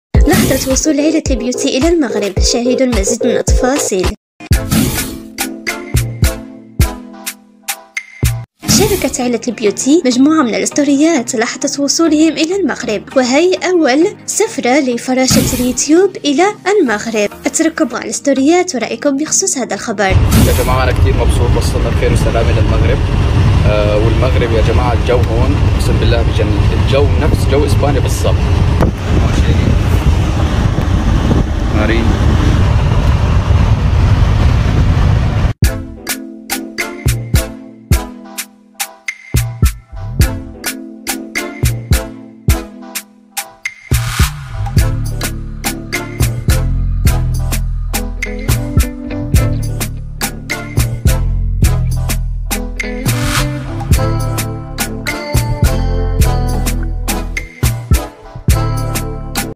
لحظة وصول نارين بيوتي وشيرين وجلال للمغرب مراكش 😍🙈
لحظة وصول نارين بيوتي وشيرين للمغرب

شكرا كثير لدعمكم ومحبتكم🙏❤ وتعليقاتكم الحلوة ما بتعرفوا اديش بكون مبسوطة☺ وانا عم شوف كل 🧐 تعليقاتكم✍️ والله كثير فخورة فيكم وكمان بتعطوني طاقة اني استمر❤🎞🎥